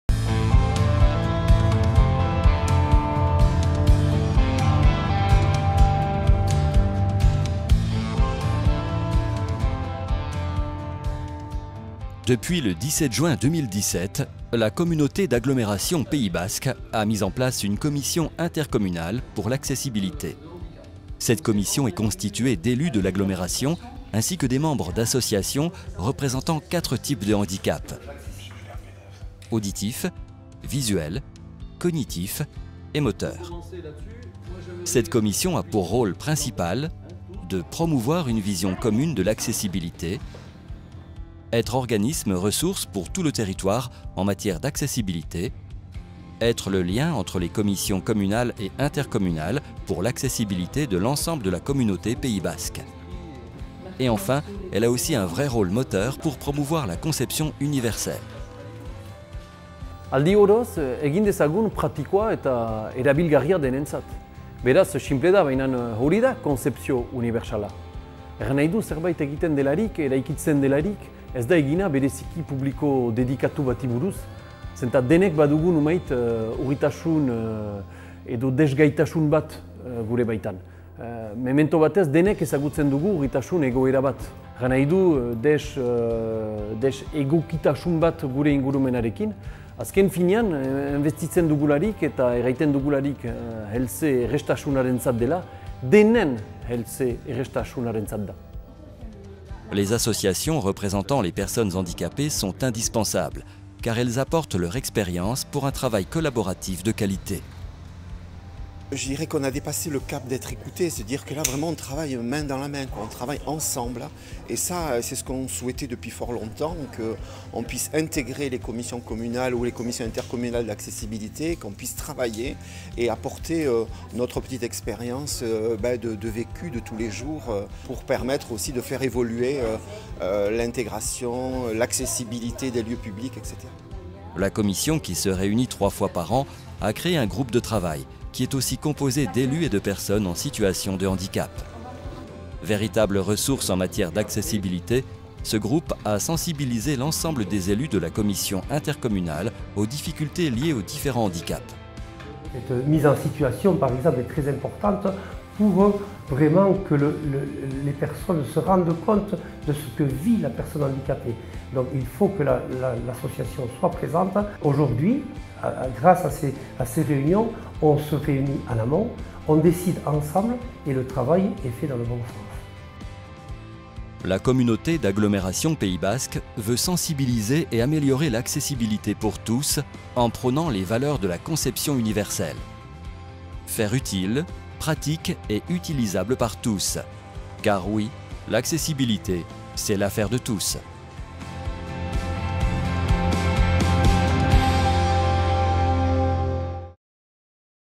Commission Accessibilité du Pays Basque. C’est quoi ?
Depuis juin 2017, la Communauté Pays Basque a mis en place une commission Accessibilité. C’est quoi ?
Vidéo langage des signes + Sous-titrée